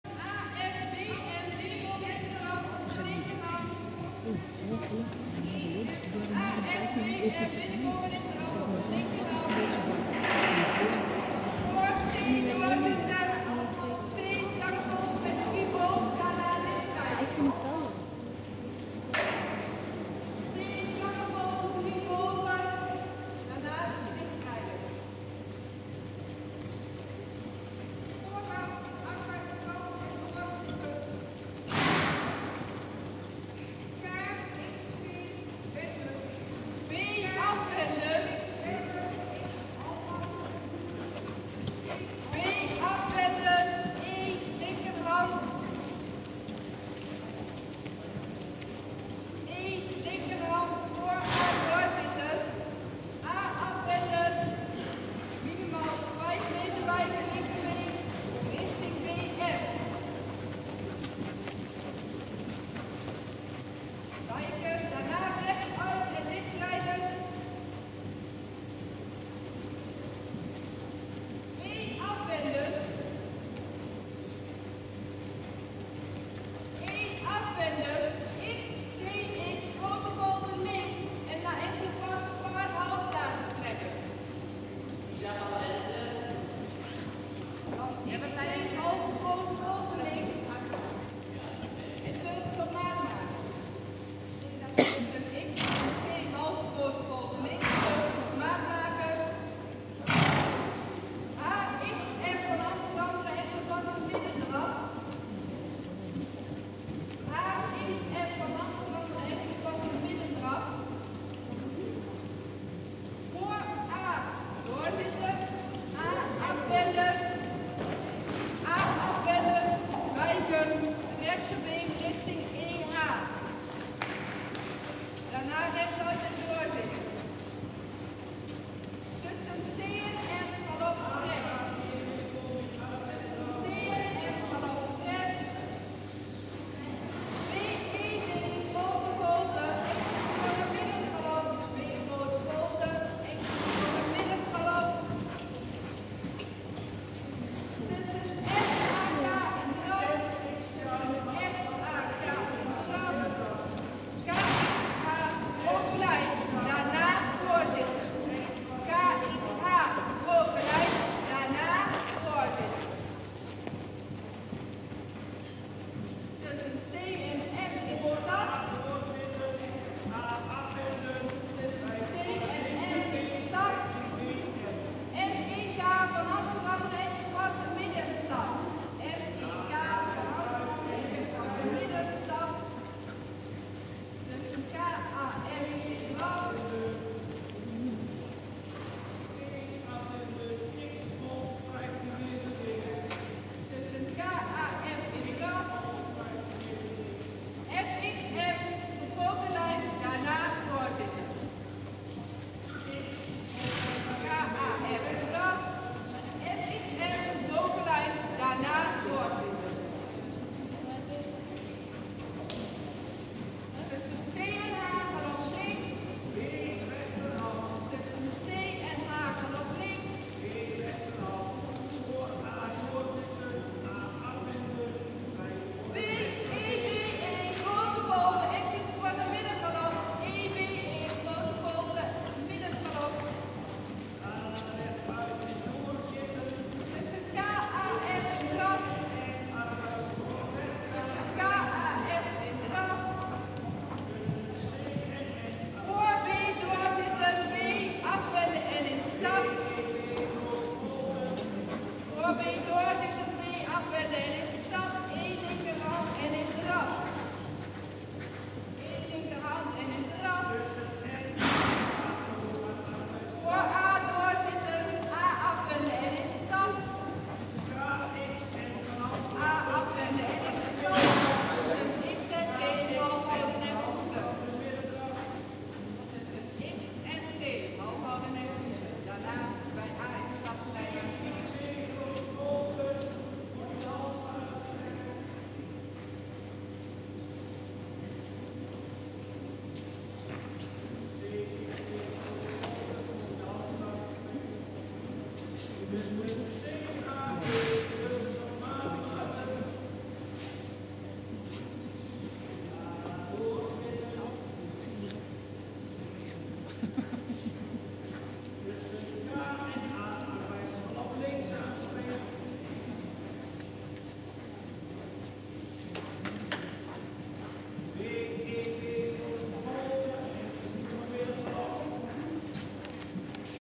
Siepke's Princess HSH met Lobke winnende L1-proef 15-12-2013
E-pony merrie Siepke's Princess HSH met Lobke tijdens de winnende L1-proef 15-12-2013 in Vragender (198 punten).